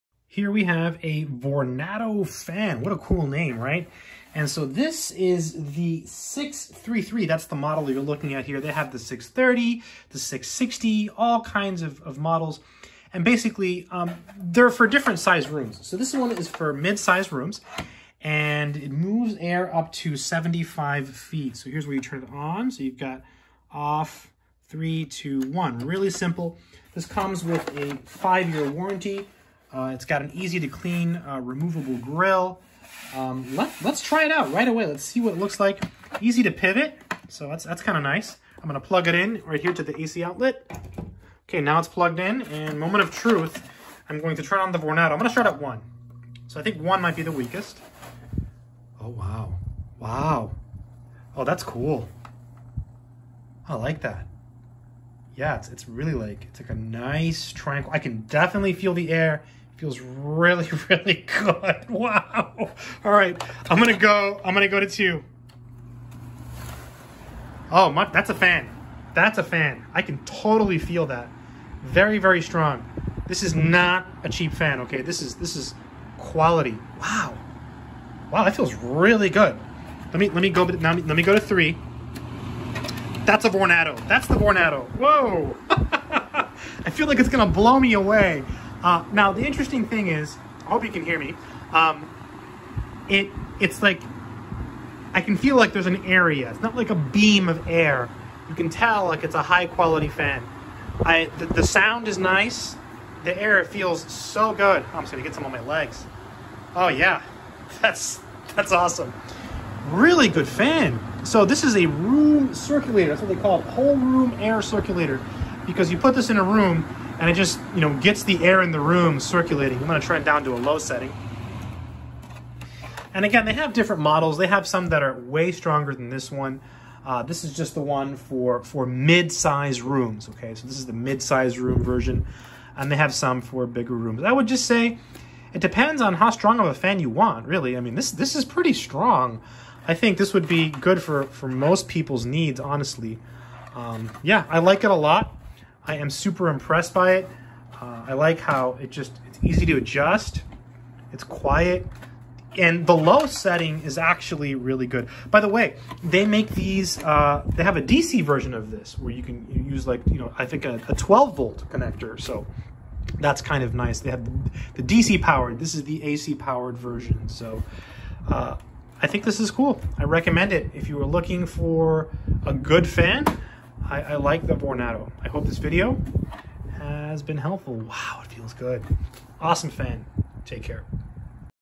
Vornado Fan Model 633
In this video I talk about a very nice fan. This is the Vornado Fan and the model is 633. This is very similar to the 630 model.

Here is one on amazon